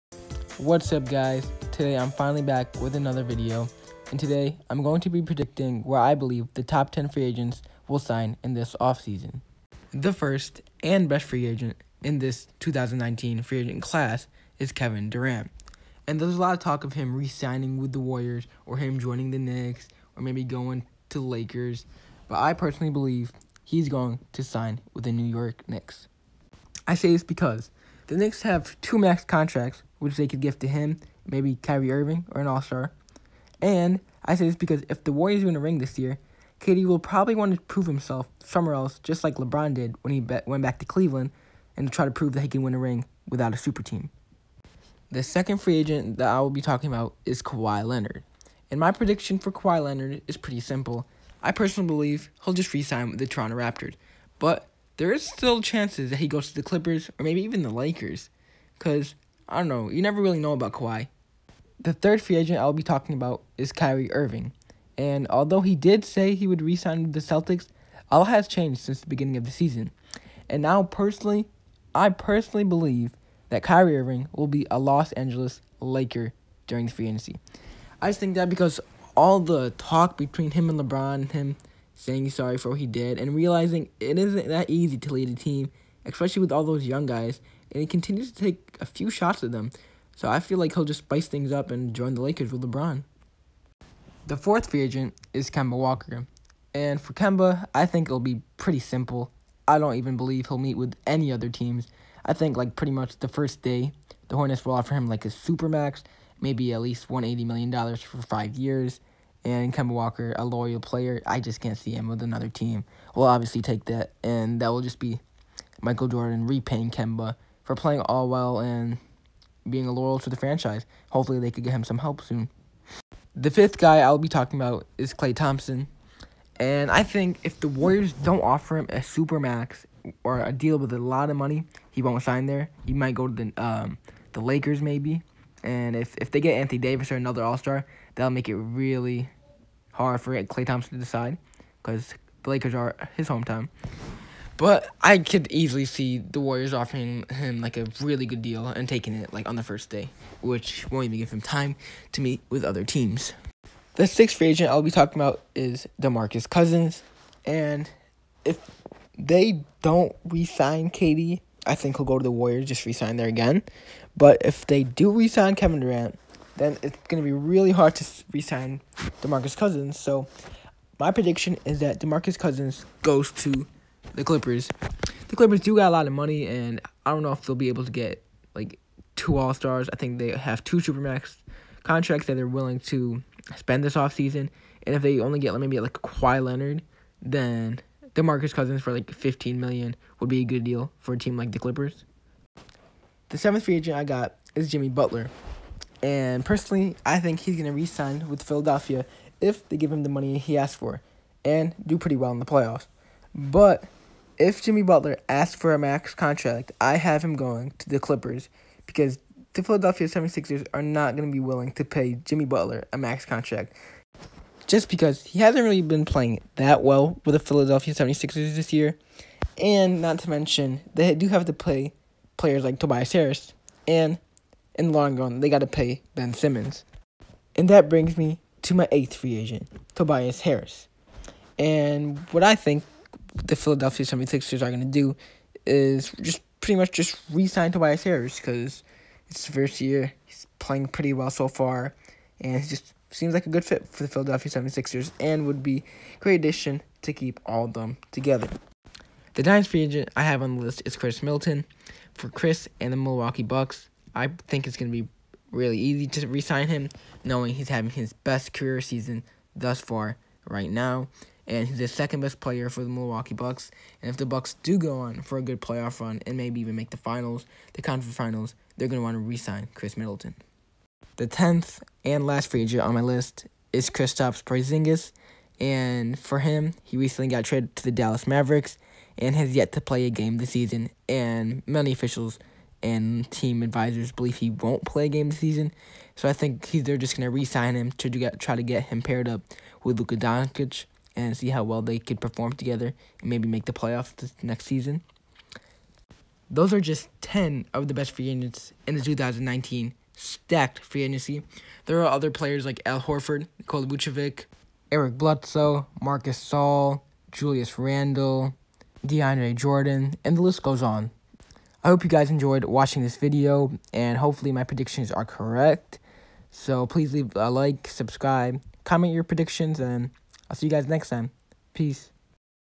PREDICTING WHERE NBA FREE AGENTS WILL SIGN THIS OFF-SEASON (2019)
My predictions of where free agents will sign this off-season